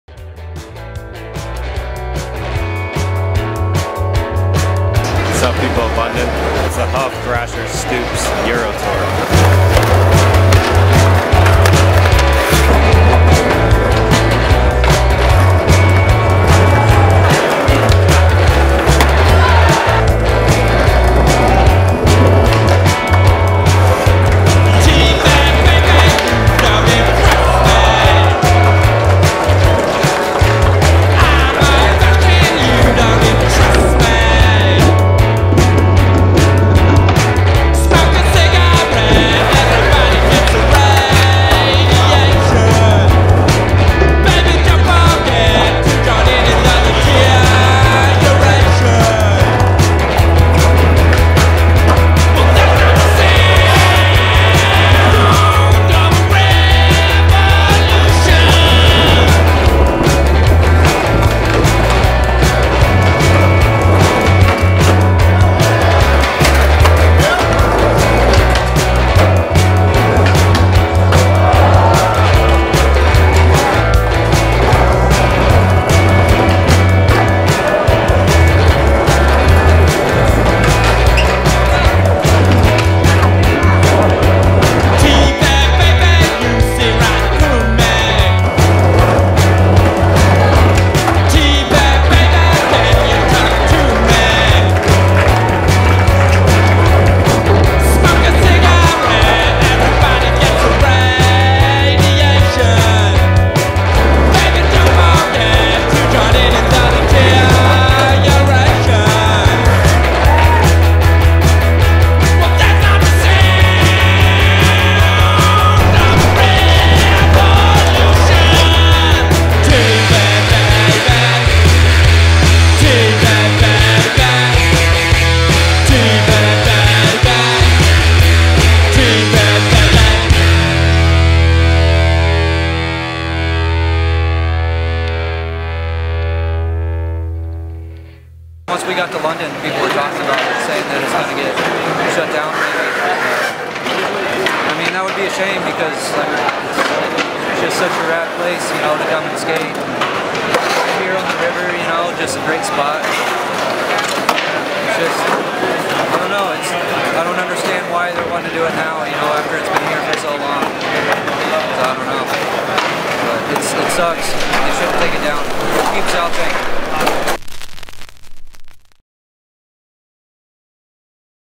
HUF Stoops Tour Demo - Southbank, London
The HUF team skate Southbank in London featuring Dylan Rieder, Austyn Gillette, Keith Hufnagel, Josh Matthews, Kevin Terpening, Brad Cromer, Dan Plunkett, Joey Pepper and Peter Ramondetta.

Filmed by Dan Joyce at O Films for the UK's #1 Skater Owned Skateboard and Music Magazine.